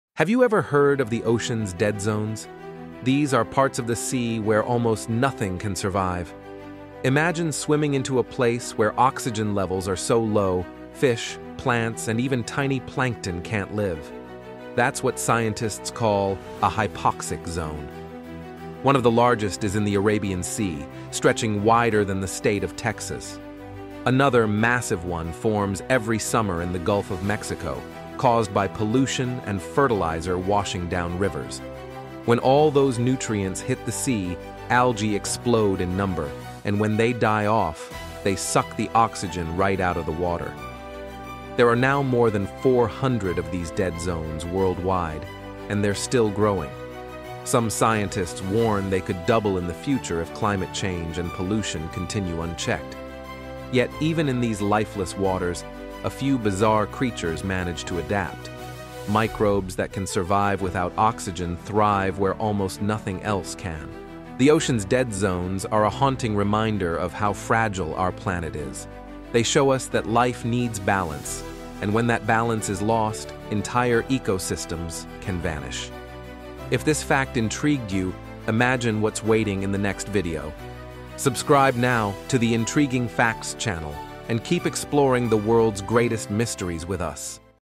The Ocean’s Dead Zones – Where Nothing Can Survive
Did you know there are places in the ocean where life simply cannot survive? These “dead zones” are spreading across our planet, caused by pollution, climate change, and nature’s delicate balance being thrown off. From the Arabian Sea to the Gulf of Mexico, discover the chilling truth about these underwater wastelands.